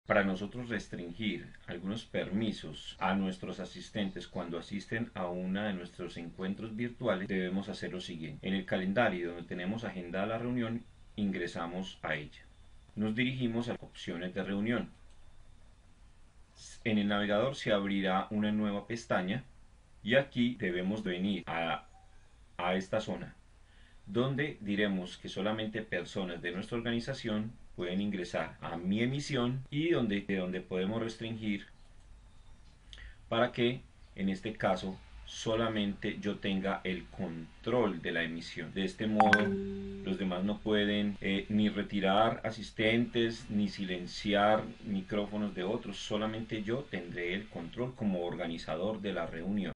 CONFIGURACION ASISTENTES TEAMS
Explicación de como se configura una reunión para que solo el docente tenga el control de la misma.